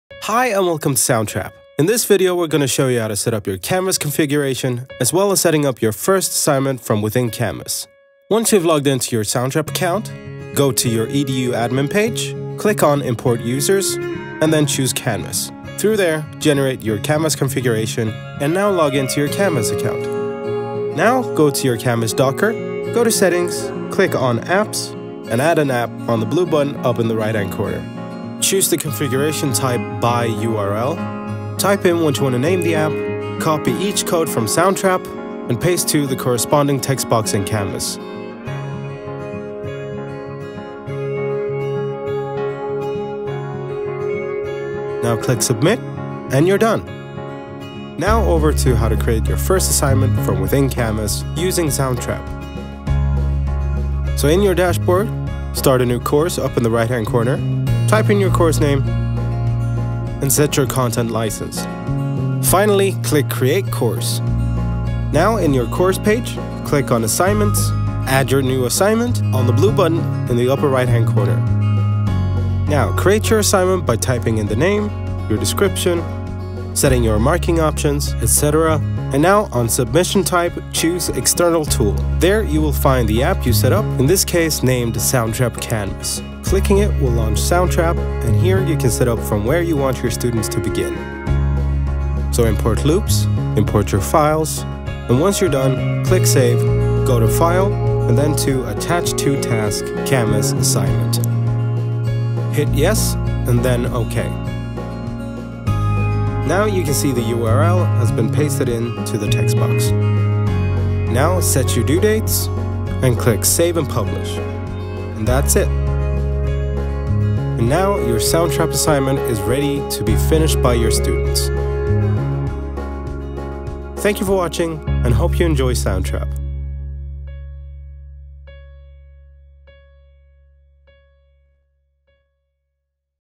Soundtrap Tutorials | Canvas integration
How to set up your Canvas integration as well as setting up your first assignment through Canvas!